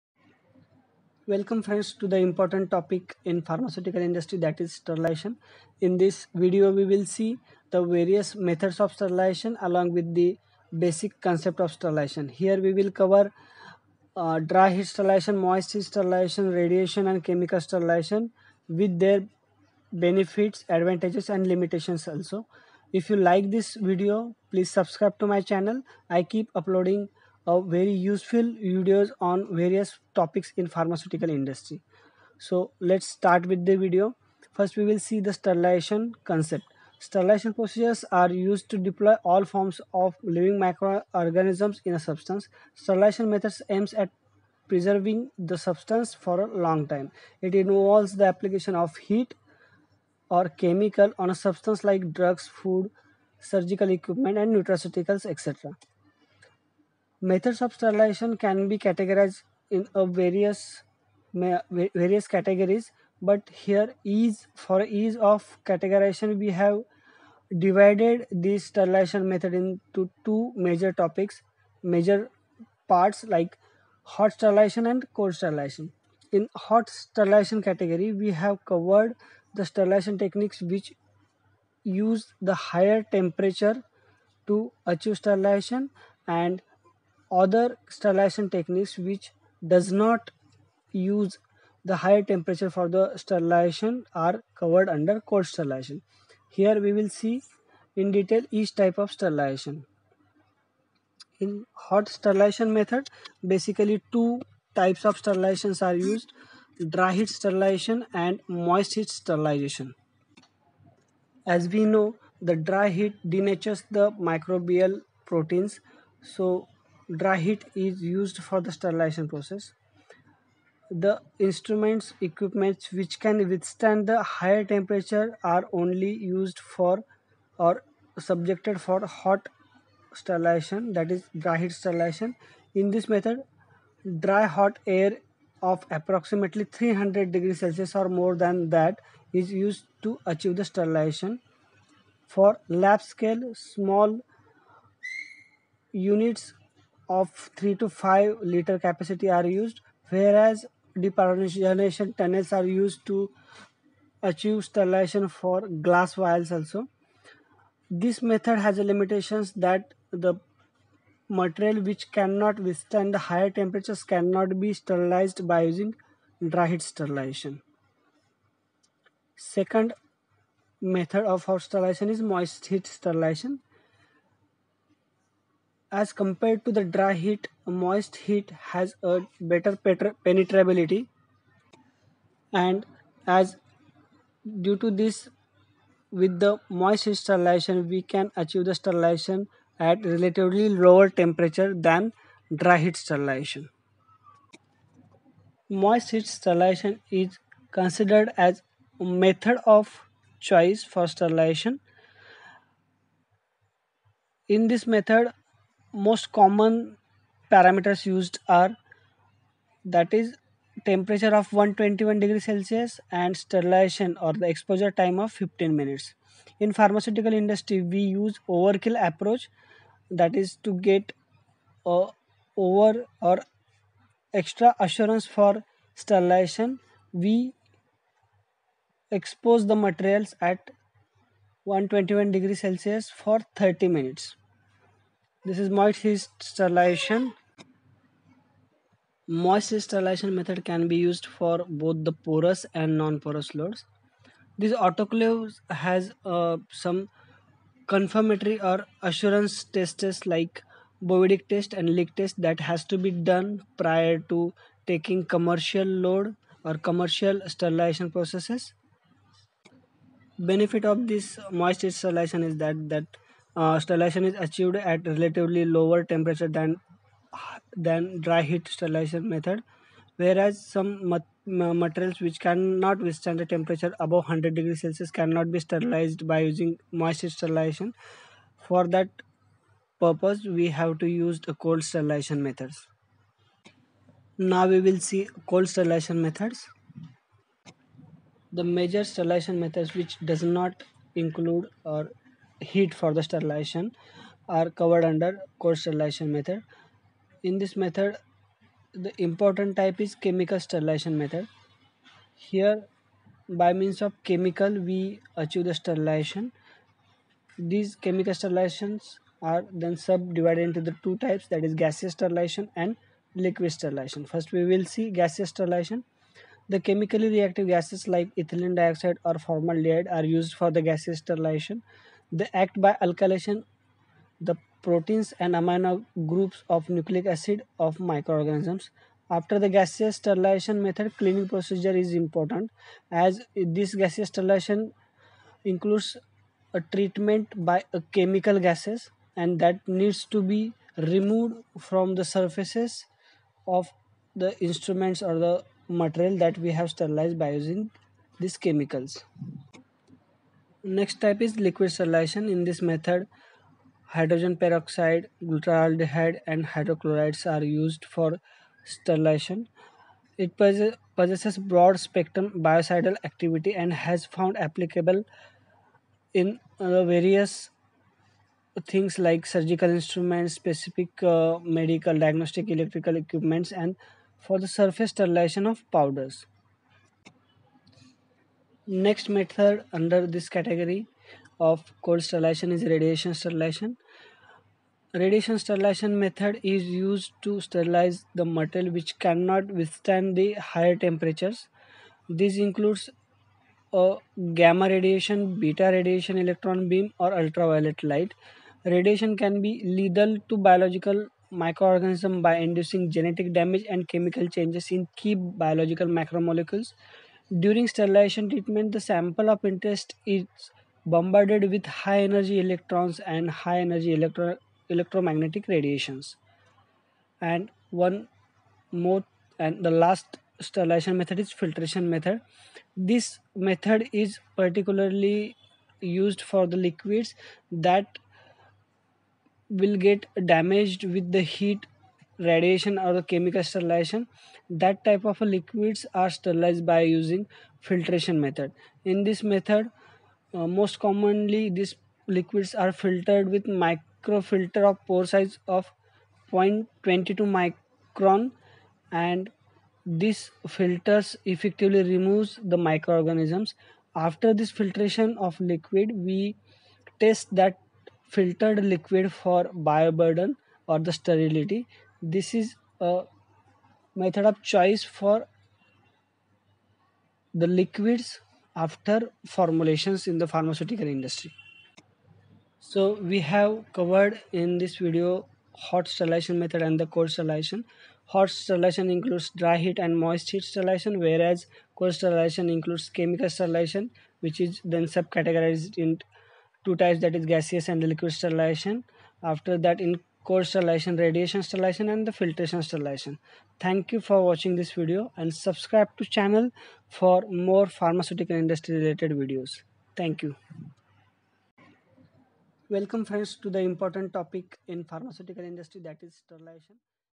Methods for Sterilization in pharmaceutical industry l Basic and important
In this video we will see basic concept, advantages and limitations for various sterilization methods deployed in pharmaceutical industry.

Methods for Sterilization are classified as below,

Dry heat sterilization
Moist heat sterilization
Chemical sterilization
Gaseous sterilization